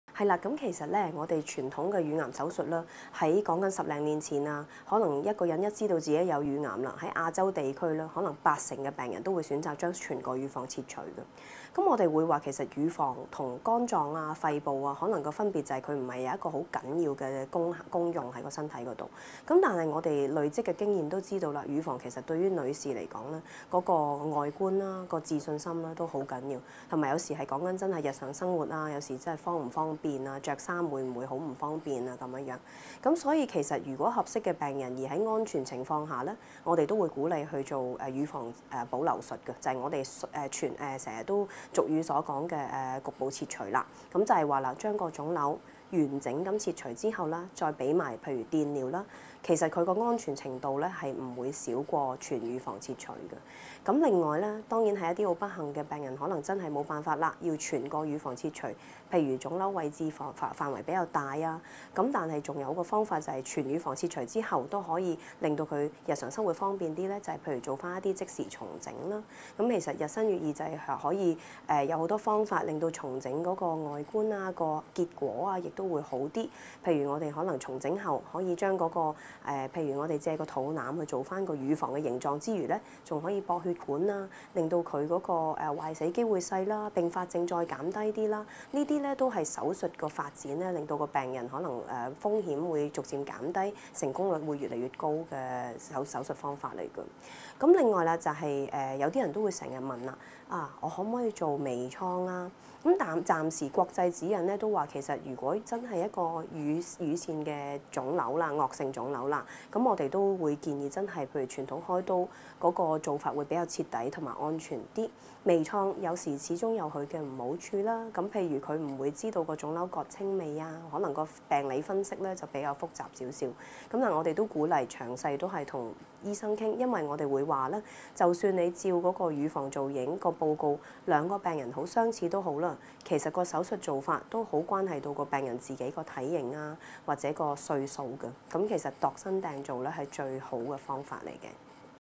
「癌病新症求助指南」乳癌患者的手術 與 乳房重建, 周芷茵醫生
醫學日新月異, 今天乳癌患者的治療方案有什麼選擇?
乳腺外科周芷茵醫生 與我們講解現況。

更多的乳癌相關資訊，可以參考癌症資訊網今年新書
「癌病新症求助指南」

現於各大書局有售。

是次銷售收益將全數撥捐